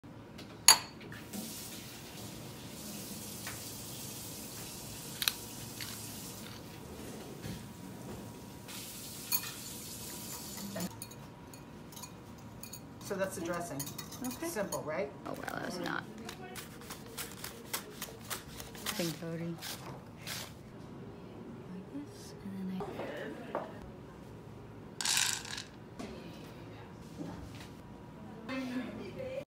Andy Boy Gone Viral Video Fennel Salad Part 1
Building the tasty fennel salad – one shave at a time.